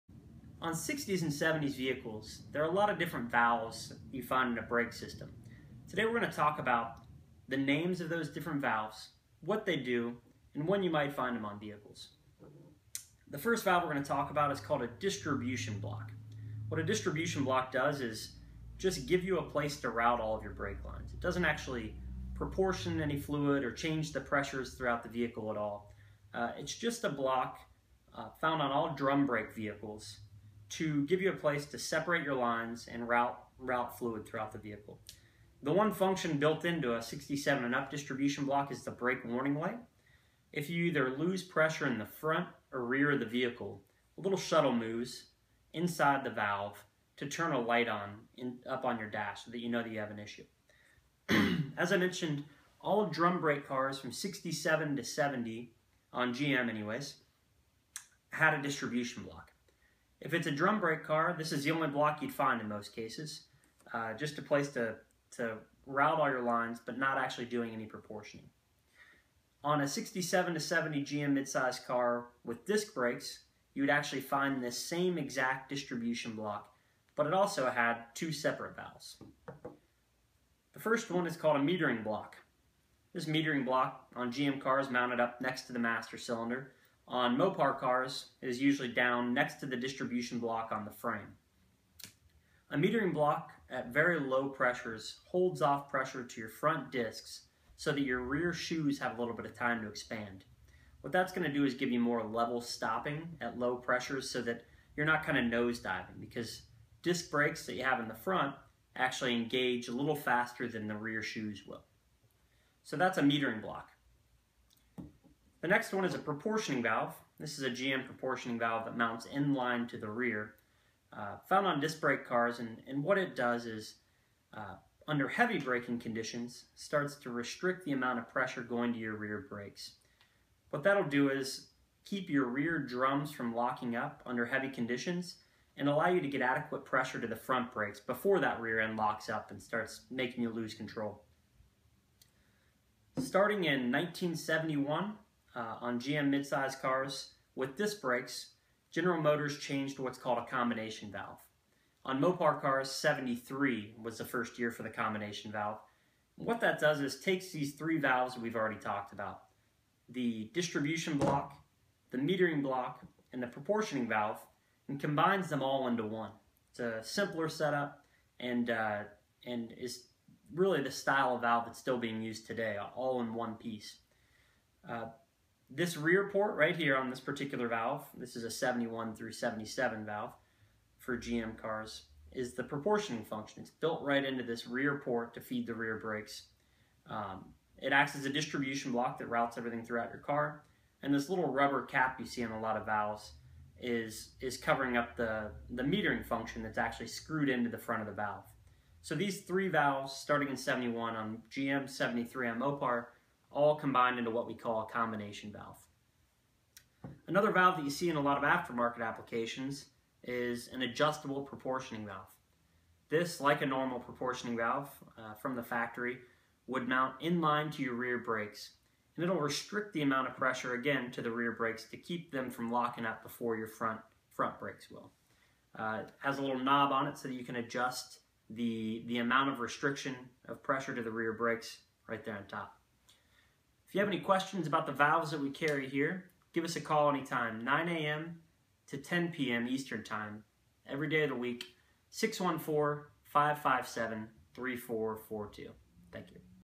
Proportioning Valves and Other Brake Blocks
Outline of brake valves found on 50s, 60s, 70s and 80s vehicles and what they do. This includes prop valves, distribution blocks, combination valves and metering blocks.